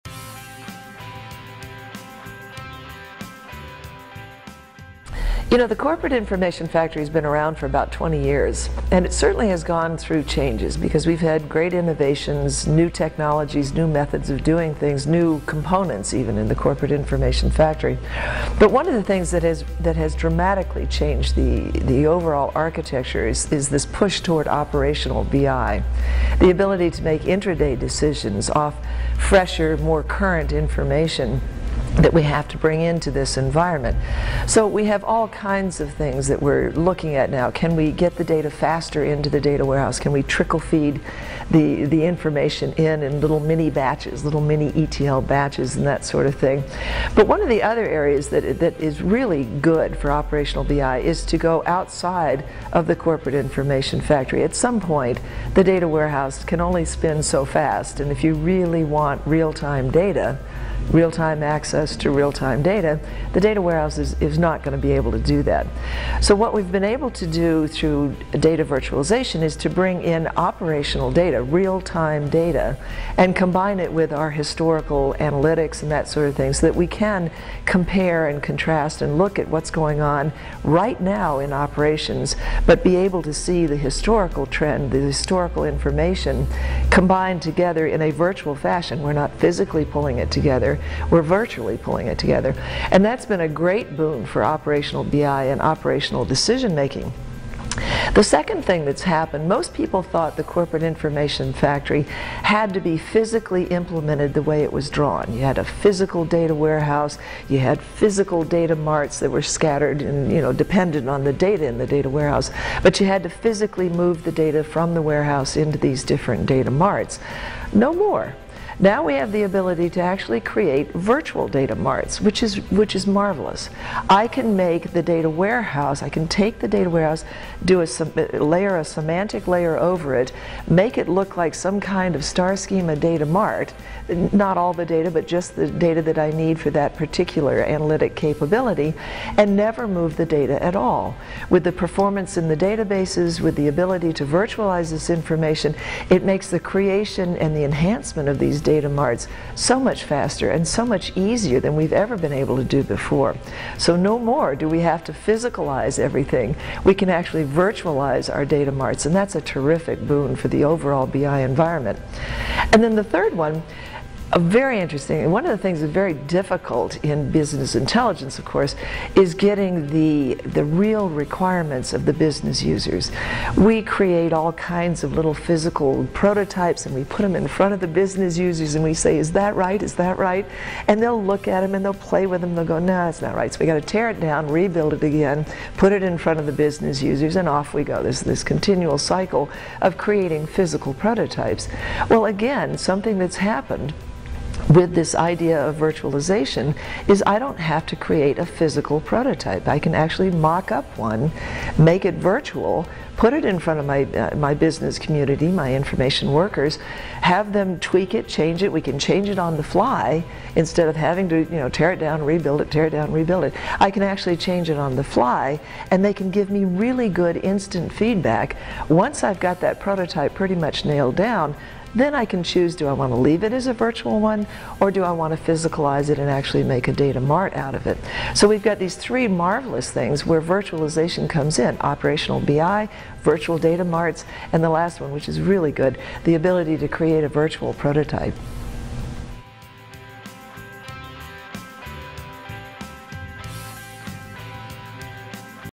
Data Virtualization: Claudia Imhoff on Updates to the Corporate Information Factory
Thoughts from Claudia Imhoff on updates to the Corporate Information Factory. Claudia explains how to gain real-time access to real-time data.

- Data Virtualization
- Corporate Information Factory
- Operational BI
- Rapid Prototyping
- Virtual Data Marts